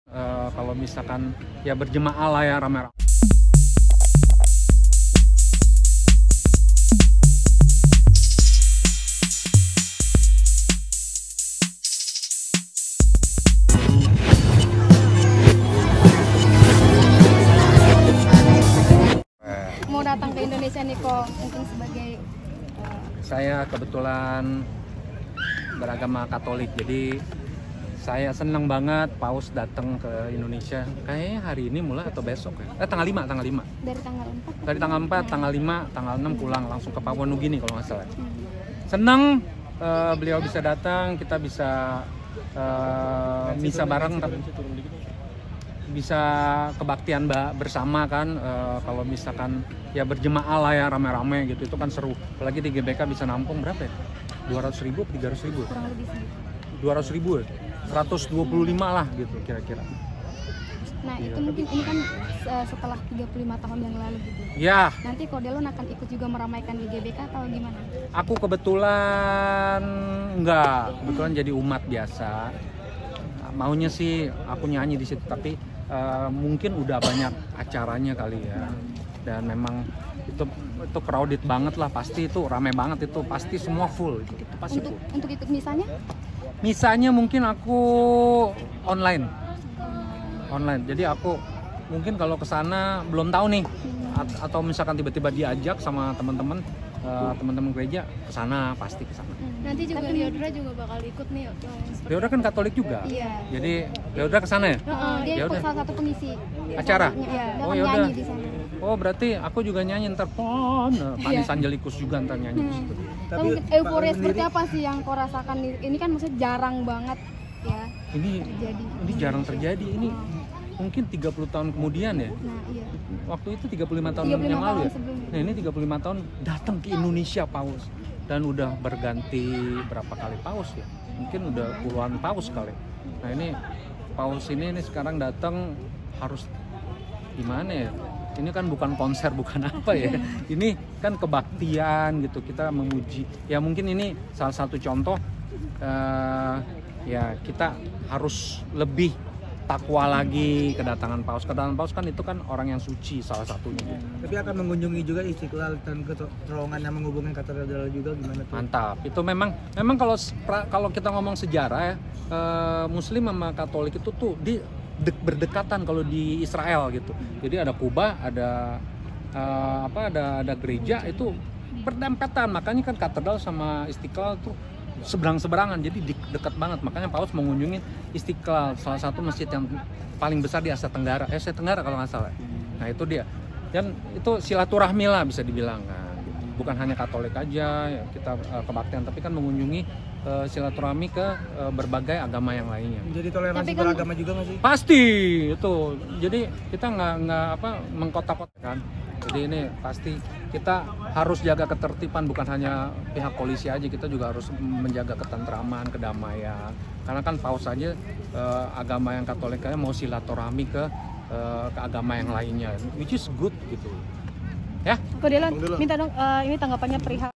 Delon Bangga dengan Kunjungan Paus Fransiskus ke Indonesia: Momen Bersejarah untuk Umat Katolik
Delon Merasa Bangga dengan Kunjungan Paus Fransiskus ke Indonesia: Momen Bersejarah untuk Umat Katolik